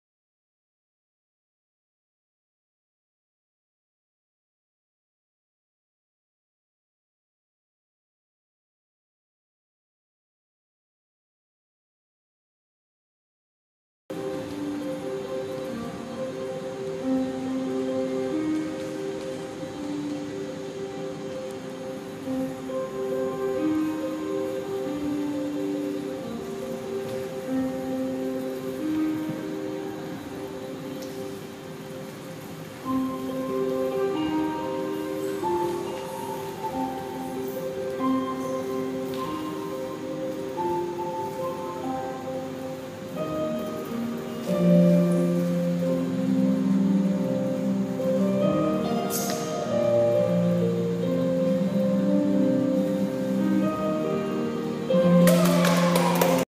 JCB Danceworks Sophia
Thank you JDB Danceworks for having me in to teach two classes at your summer intensive. This young dancer Sophia was a real treat to have in class.